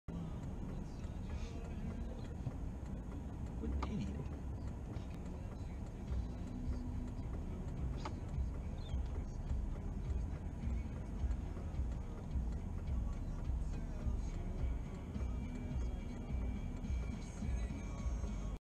Moron in red Toyota shoves himself into the line
2/22/2019 Worcester Ma. Idiots always so this shit in this spot. Too impatient to wait in line. They shoot up the side then try to force their way in.